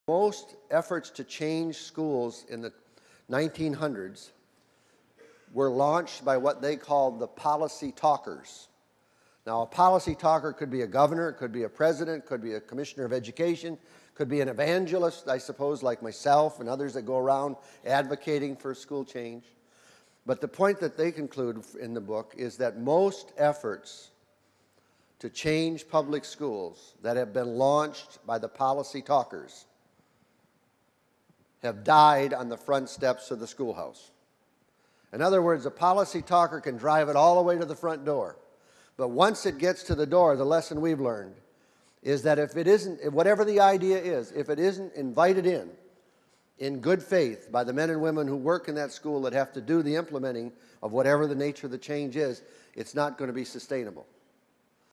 Larry Lezotte Keynote--Effective Schools Conference
A one-minute clip from Larry Lezotte's keynote at the 24th Annual Effective Schools Conference.